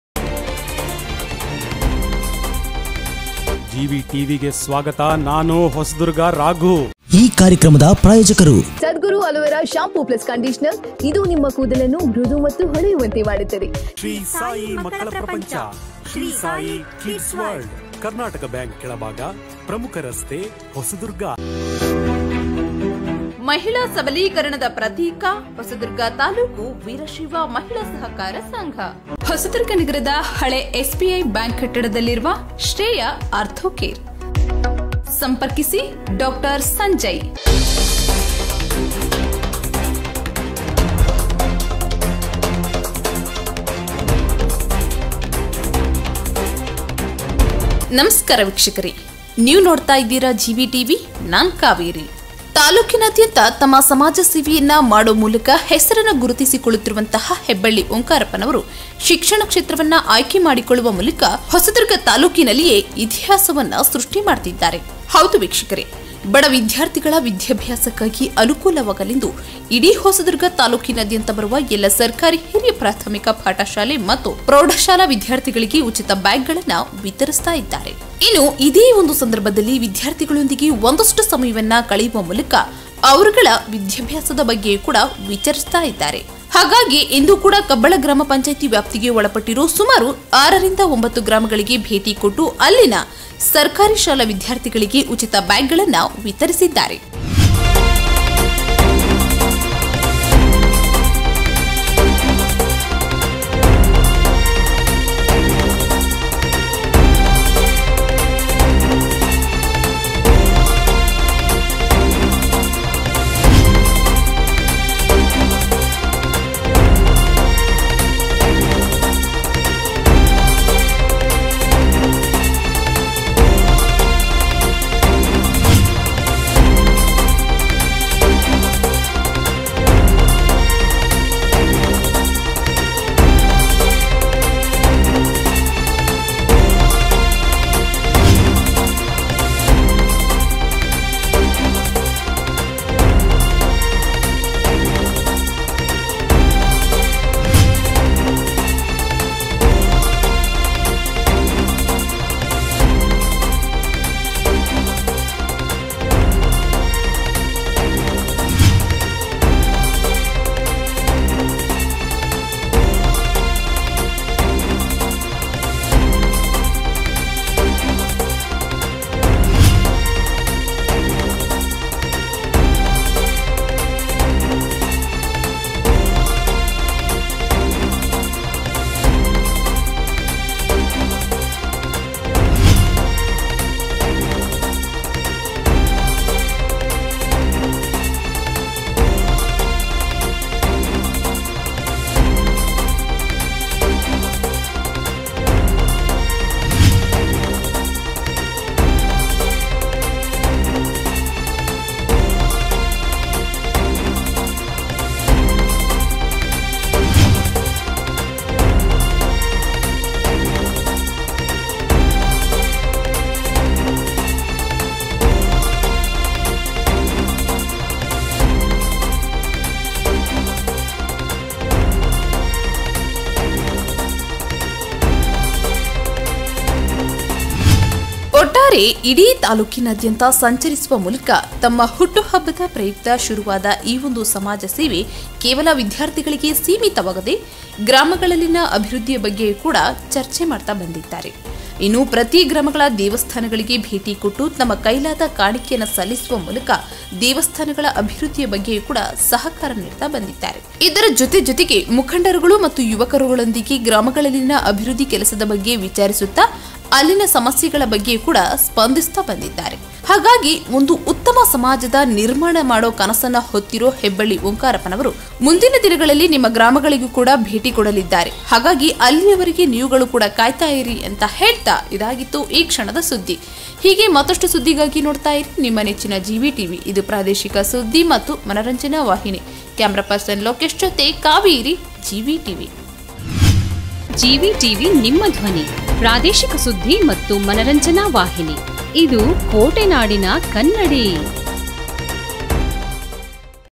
ಕಬ್ಬಳ ಗ್ರಾ.ಪಂ.ವ್ಯಾಪ್ತಿಯ ಶಾಲಾ ವಿದ್ಯಾರ್ಥಿಗಳಿಗೆ ಉಚಿತ"ಸ್ಕೂಲ್ ಬ್ಯಾಗ್"ವಿತರಿಸಿದ"ಹೆಬ್ಬಳ್ಳಿ ಓಂಕಾರಪ್ಪ".GVTV
ಕಬ್ಬಳ ಗ್ರಾಮ ಪಂಚಾಯತಿ ವ್ಯಾಪ್ತಿಯಲ್ಲಿ ಬರುವ ಸುಮಾರು 6 ರಿಂದ 9 ಗ್ರಾಮಗಳ ಸರ್ಕಾರಿ ಶಾಲೆಗಳಿಗೆ ಭೇಟಿ ನೀಡಿ ವಿದ್ಯಾರ್ಥಿಗಳಿಗೆ ಉಚಿತ ಸ್ಕೂಲ್ ಬ್ಯಾಗ್ ವಿತರಿಸಿ,ಗ್ರಾಮದ ಮುಖಂಡರುಗಳ ಜೊತೆ ಸಮಾಲೋಚನೆ ಮಾಡುವ ಮೂಲಕ ಅಭಿವೃದ್ಧಿ ಕಾಮಗಾರಿಗಳ ಬಗ್ಗೆ ಚರ್ಚಿಸಿದ ಸಮಾಜ ಸೇವಕ ಹೆಬ್ಬಳ್ಳಿ ಓಂಕಾರಪ್ಪ.